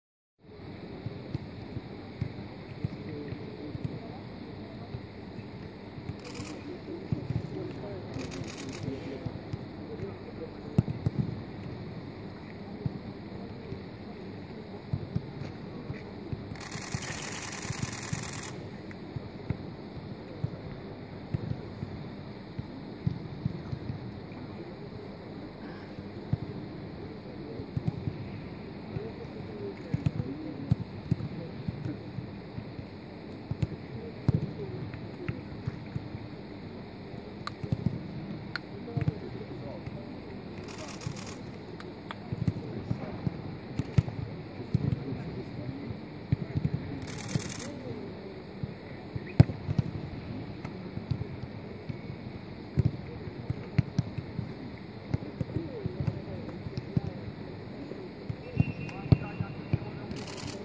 Милевский тренируется на поле "Олимпийского". Часть II
Артем Милевский тренируется на поле НСК "Олимпийский" в футболке сплитского "Хайдука" / Artem Milevskiy trains at the pitch of NSC Olimpiisky in the HNK Hajduk Split jersey, Part II